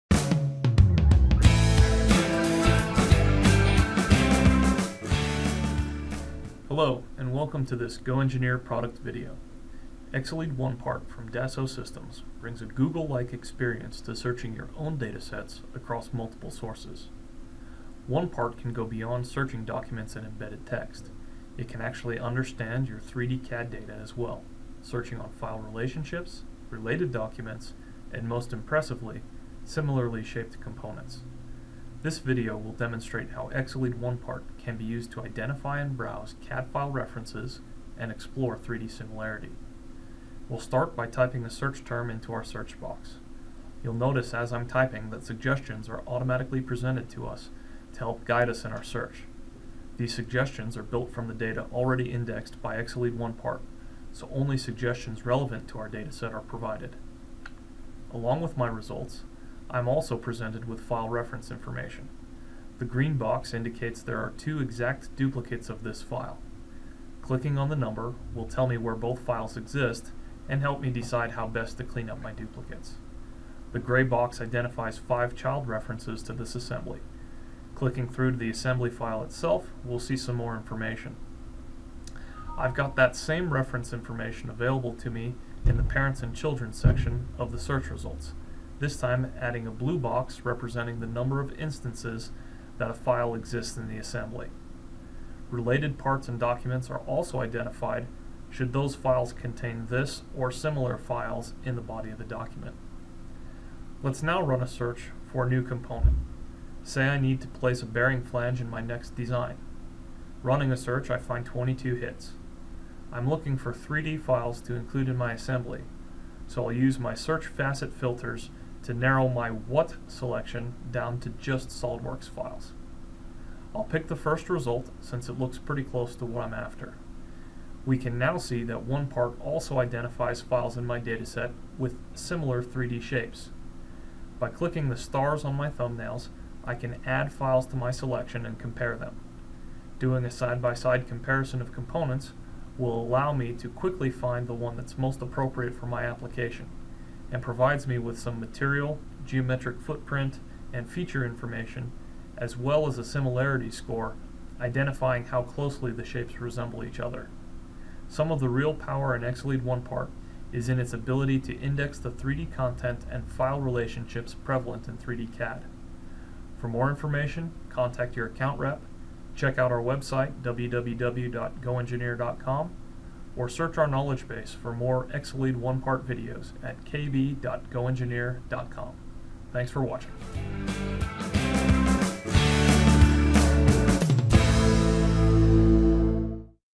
EXALEAD OnePart - Searching File References and 3D Similarity
This video is a first look at some of the reference and 3D Similarity capabilities in the EXALEAD OnePart search tool.